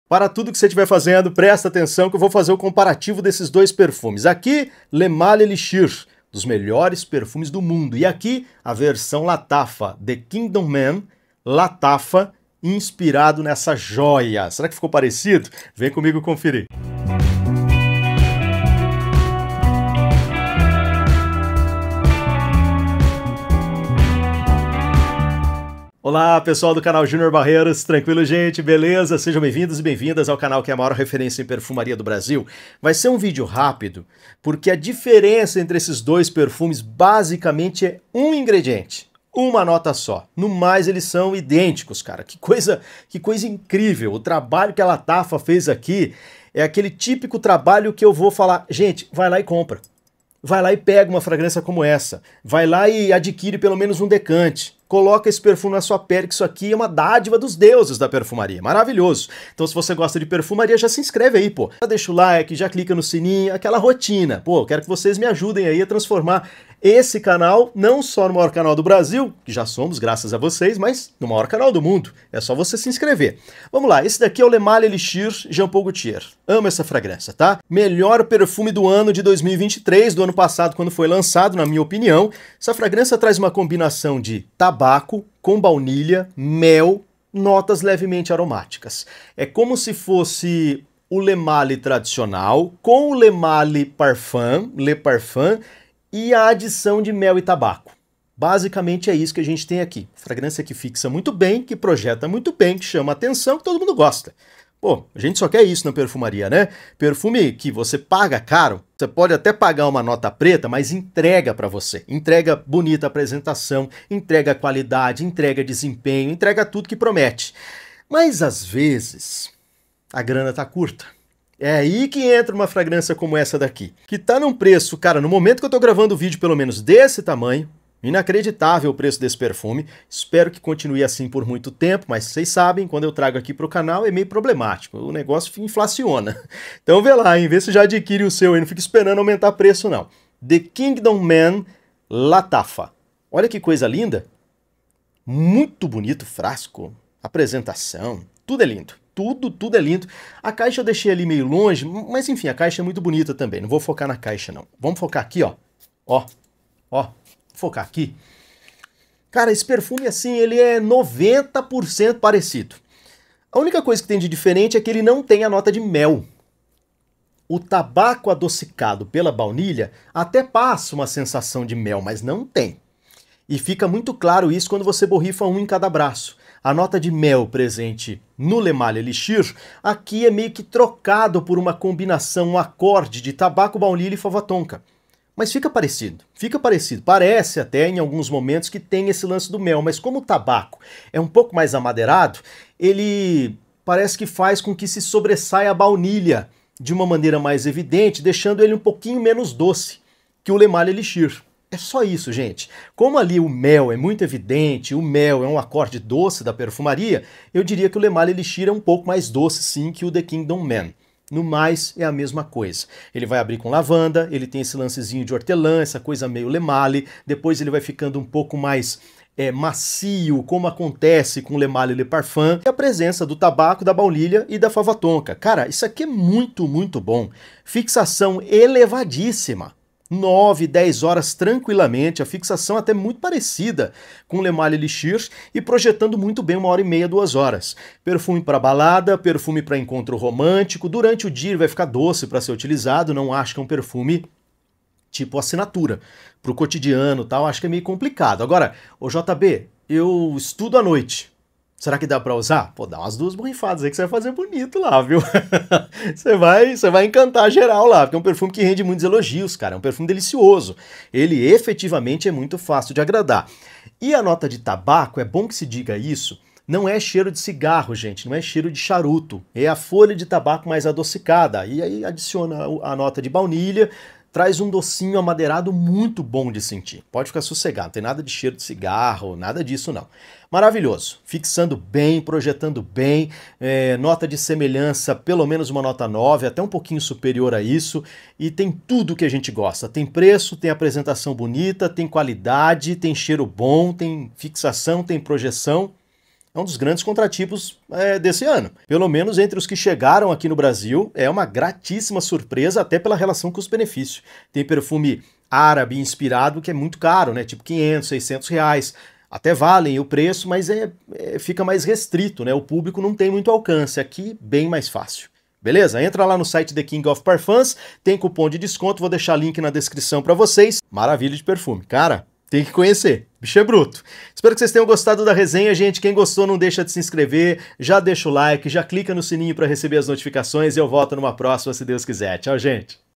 PARA TUDO PQ O LE MALE ELIXIR TEM UM CLONE ÁRABE COM PREÇO MUITO MENOR!
Resenha THE KINGDOM MAN de LATTAFA